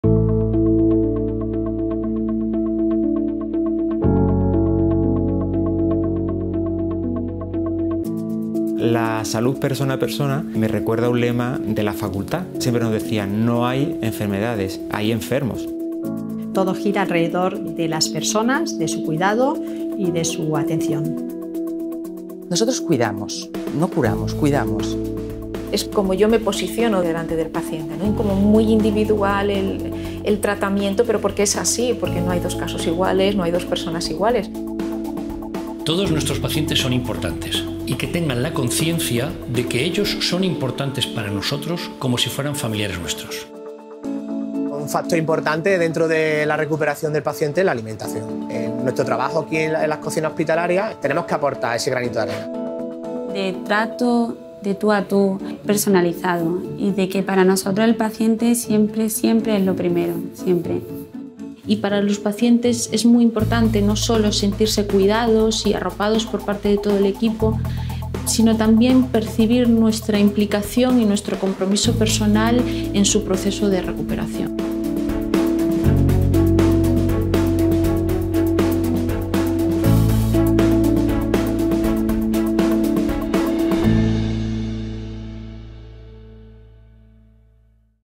ADN Quirónsalud: 7 de abril, Día Mundial de la Salud
En Quirónsalud trabajan más de 40.000 profesionales con un mismo propósito: velar por la salud de nuestros pacientes. Cada uno desde su actividad hacen todo lo posible para garantizar los mejores cuidados. Porque lo llevan en su ADN. Hoy 7 de abril, DíaMundialDeLaSalud, y los otros 364 días también.